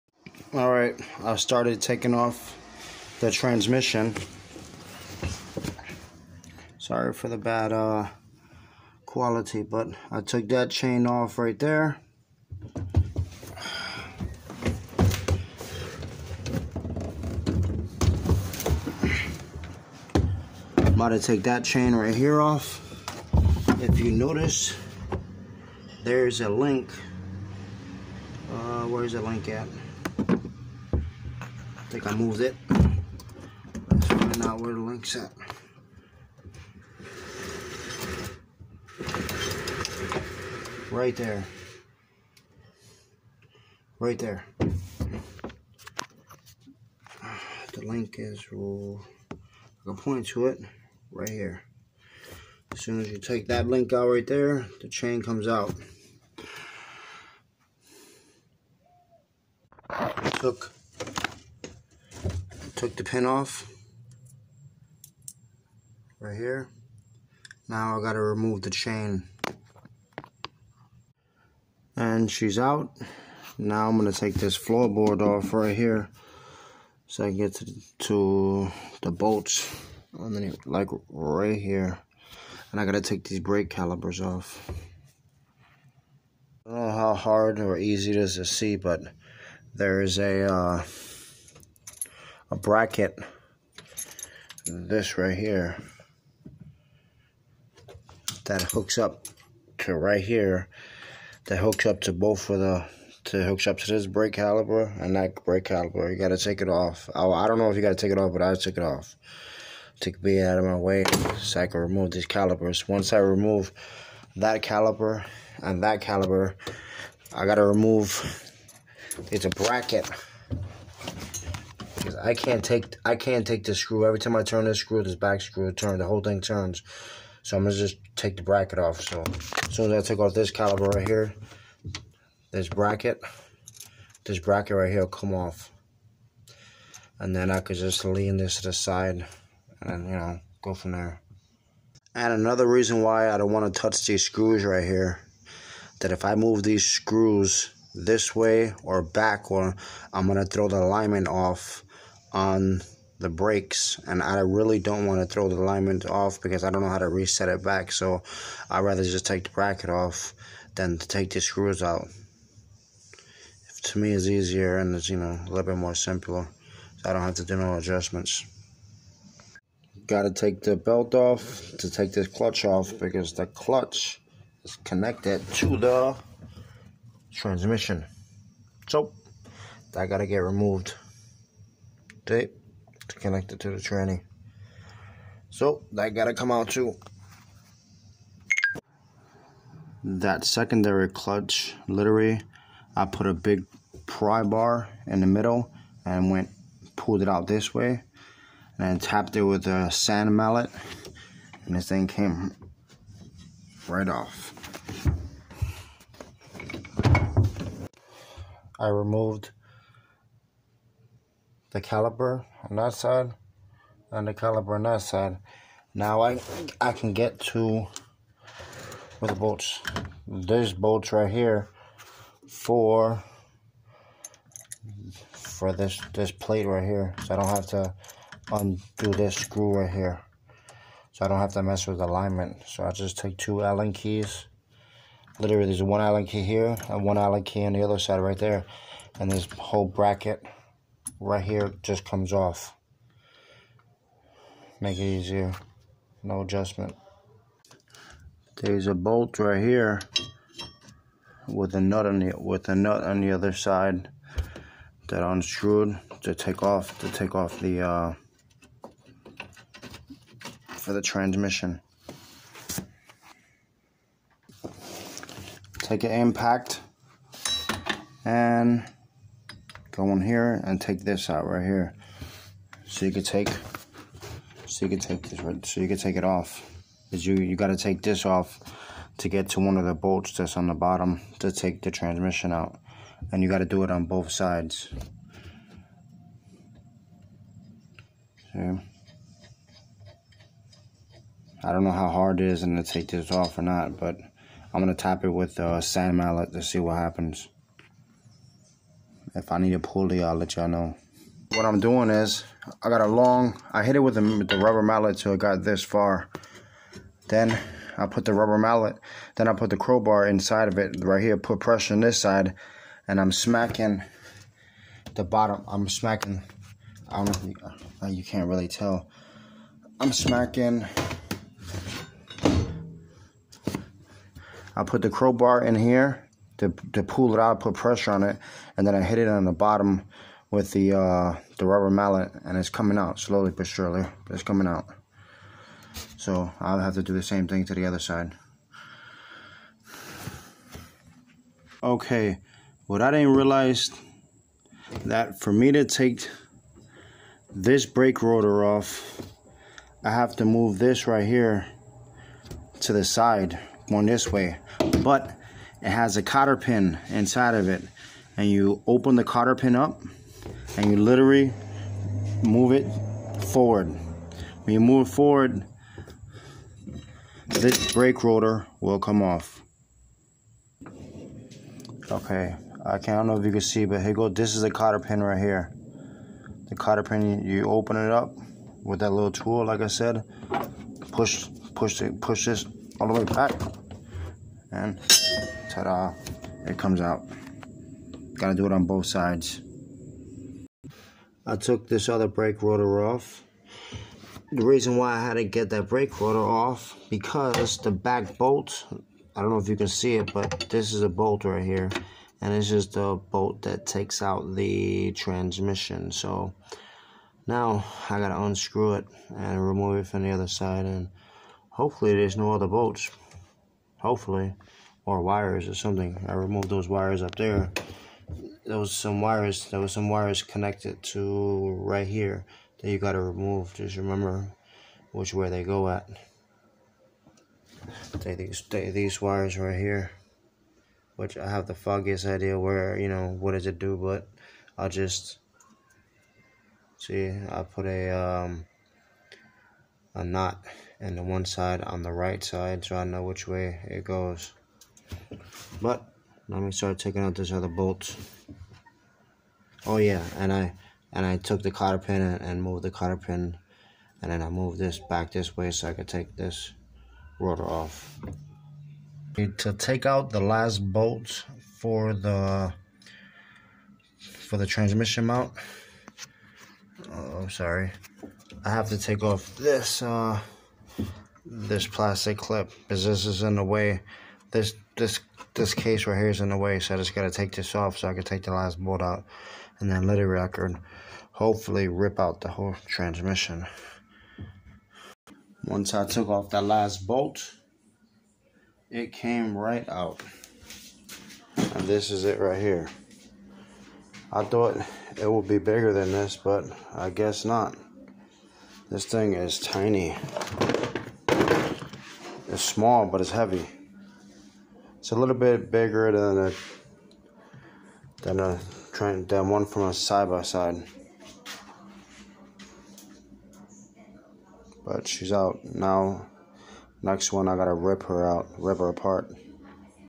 2007 Argo 8x8 avenger EFI how to remove transmission
2007 Argo 8x8 avenger EFI how to remove transmission part 1 part 2 is coming soon how to rebuild transmission